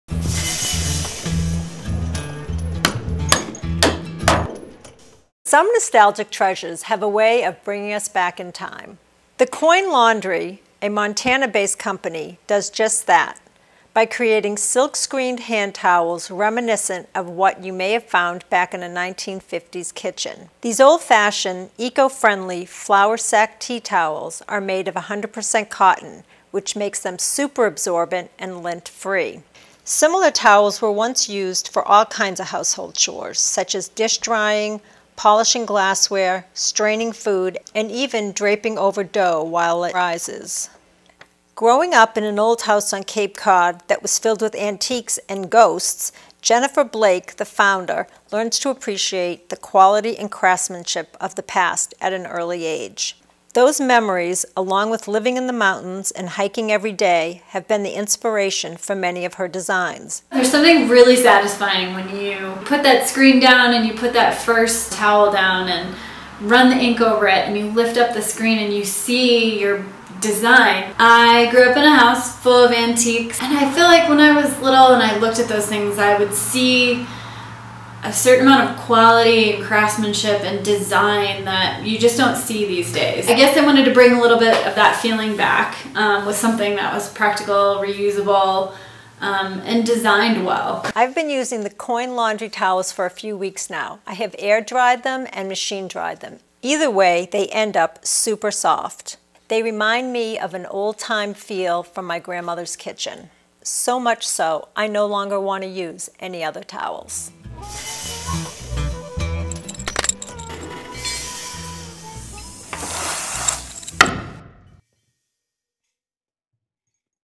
Coin Laundry - Hand Printed Flour Sack Towels
Flour sack towels by The Coin Laundry discovered by The Grommet. Shop an assortment of towels that absorbs whatever you dish out while adding a touch of 1950s retro-style to any kitchen, bathroom, or bar.